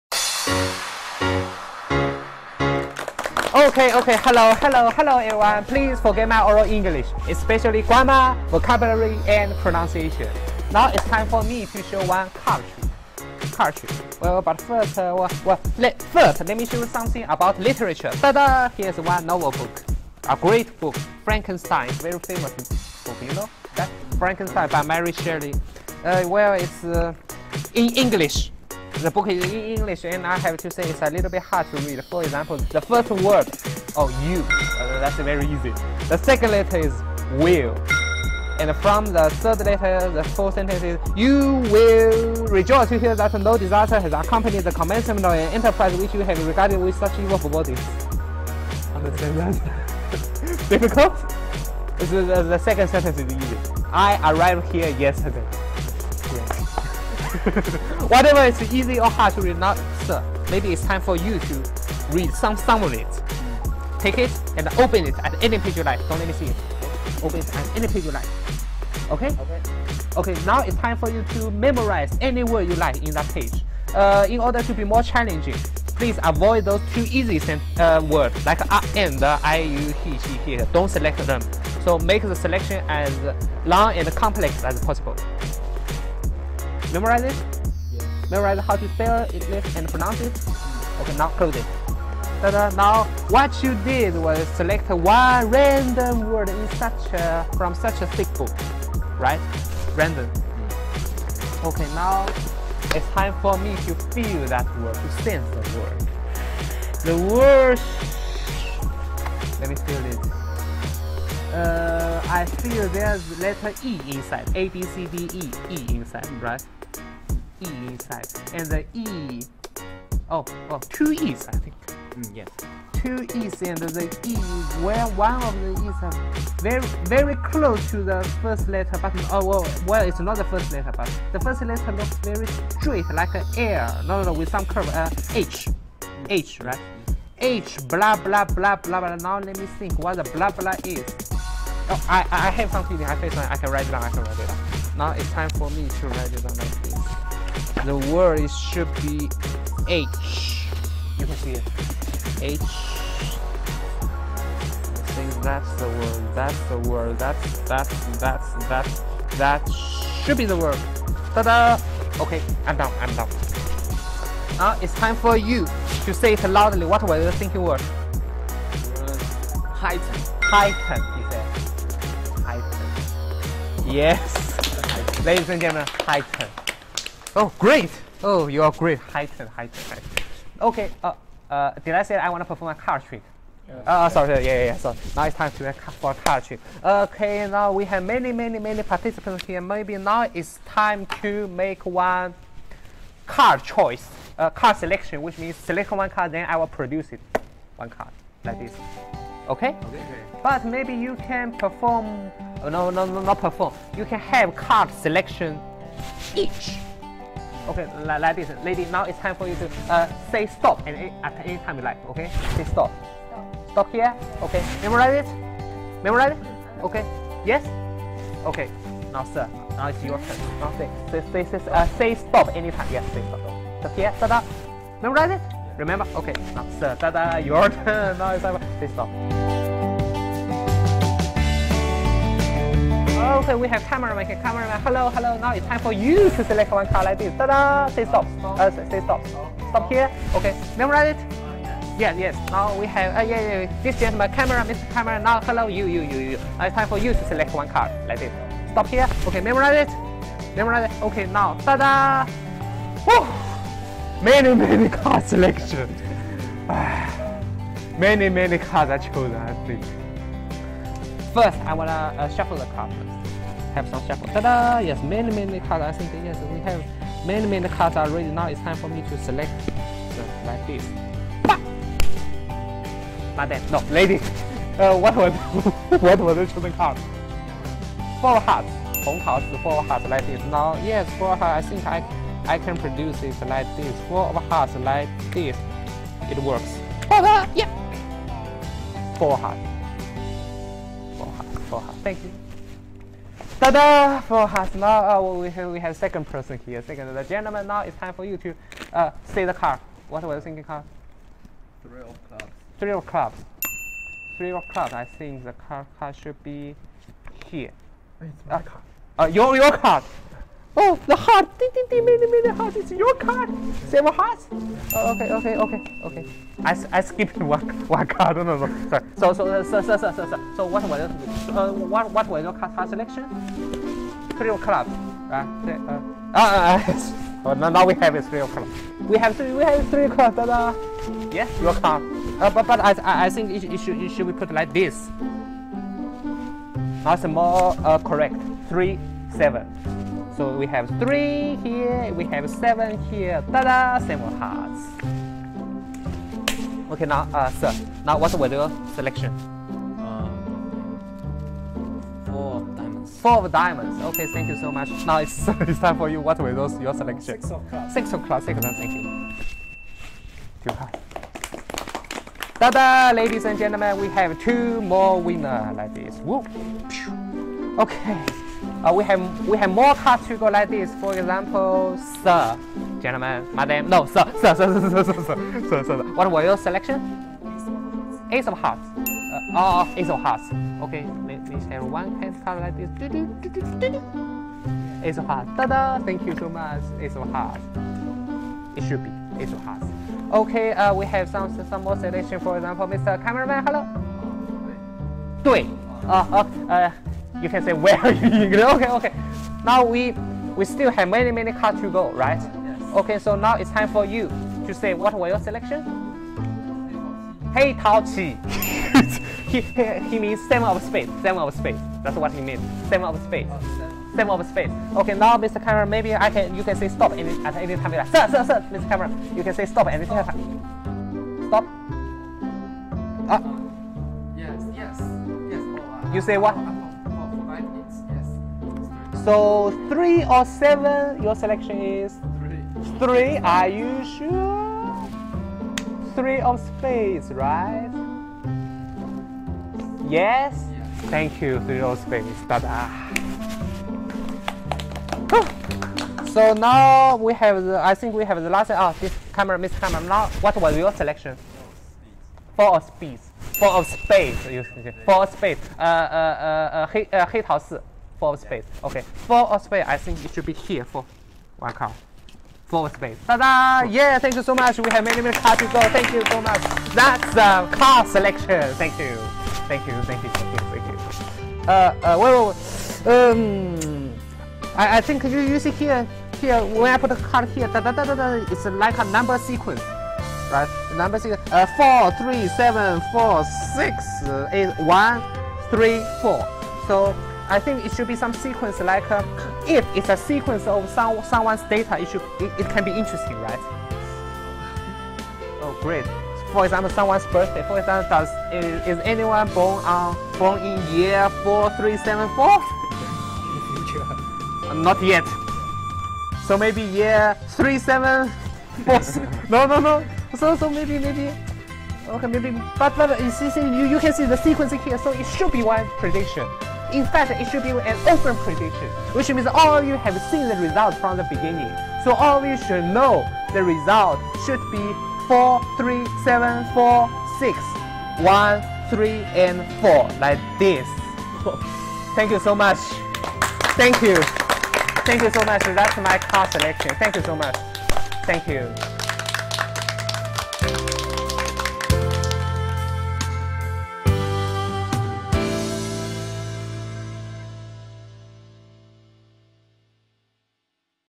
"The Prefix" Demo with Cards
My new attempt of magic, with the combination of book test and card trick. Hope you all like it.